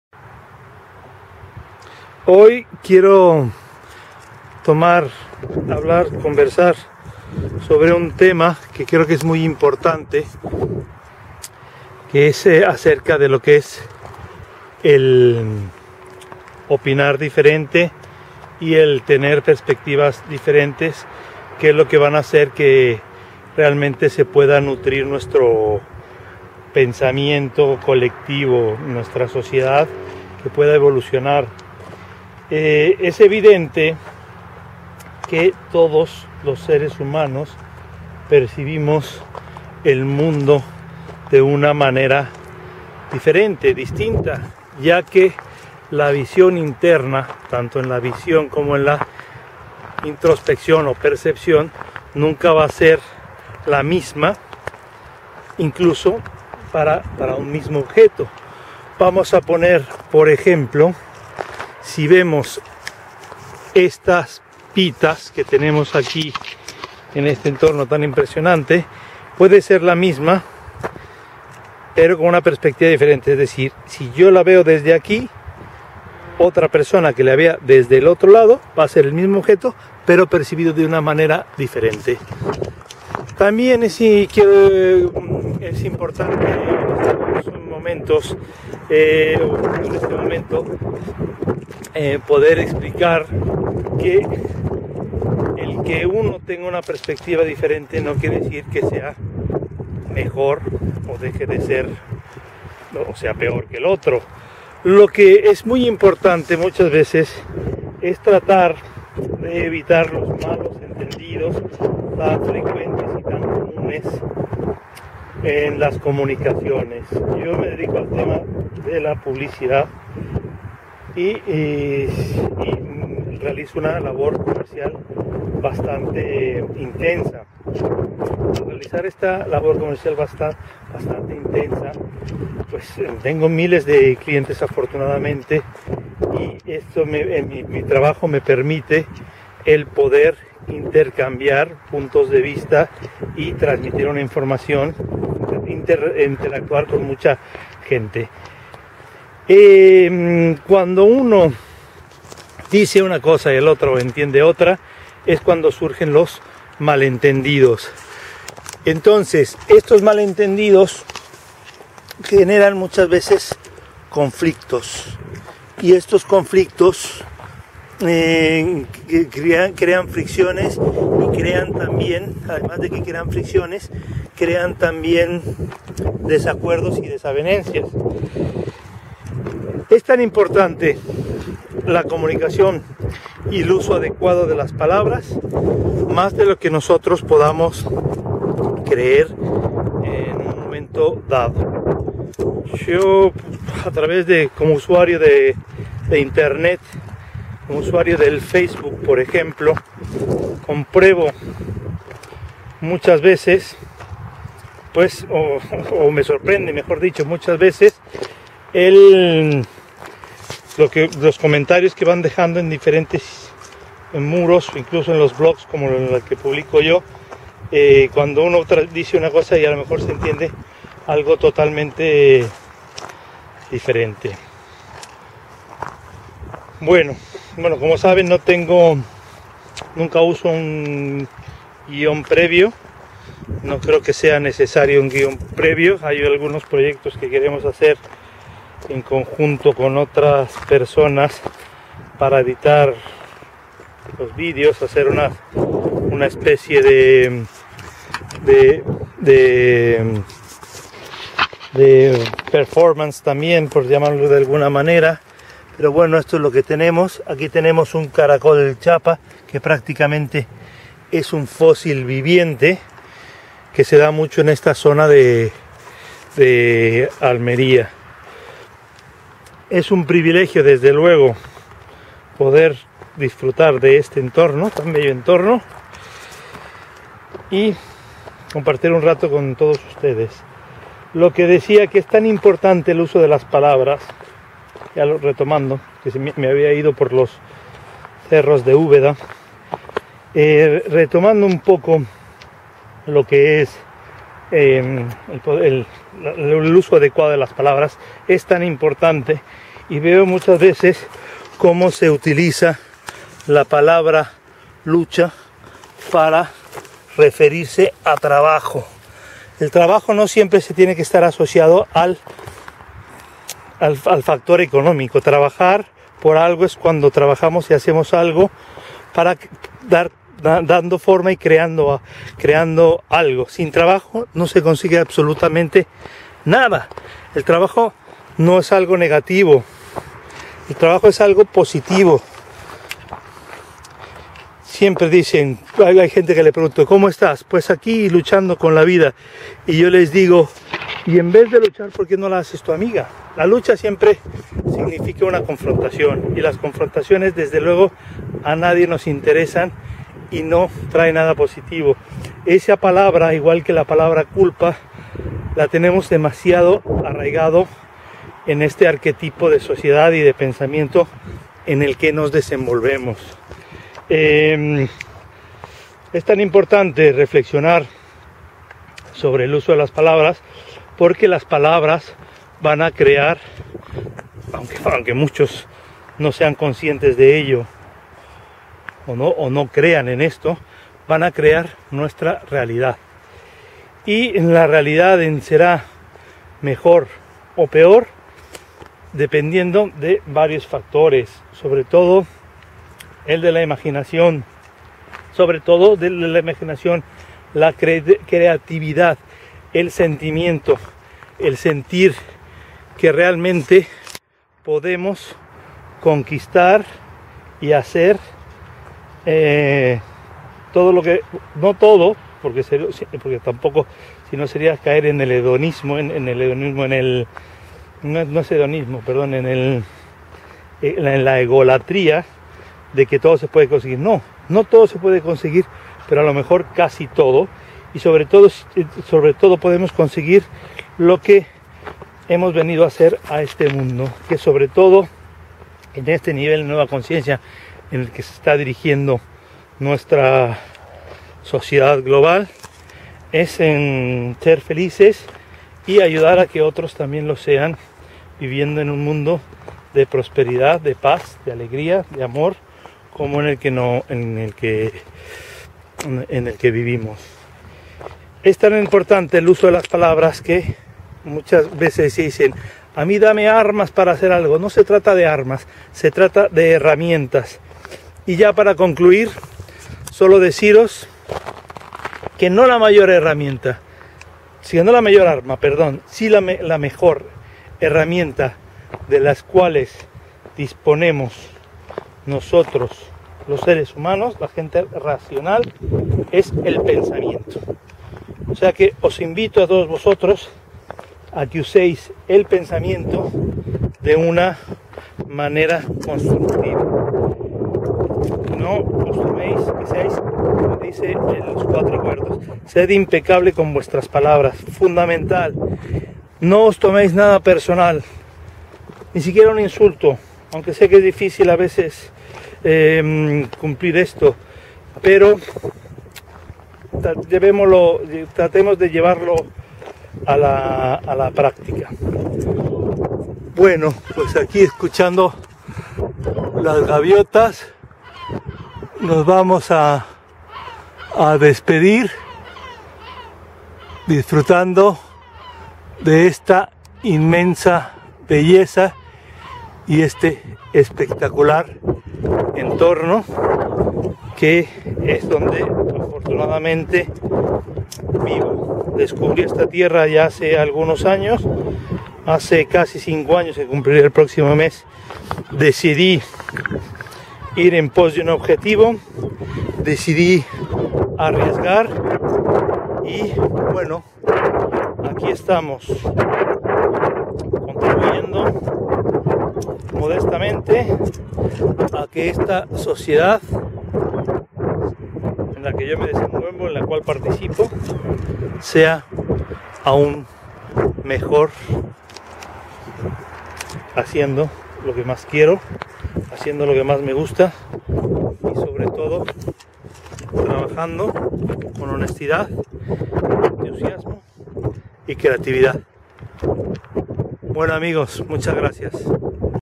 La importancia de una buena comunicación y sus palabras.
Que importante es transmitir bien una comunicación, independientemente de la perspectiva individual, para evitar malos entendidos. Es primordial ser impecable con nustras palabras y no tomarse el punto de vista del otro como un agravio y de manera personal.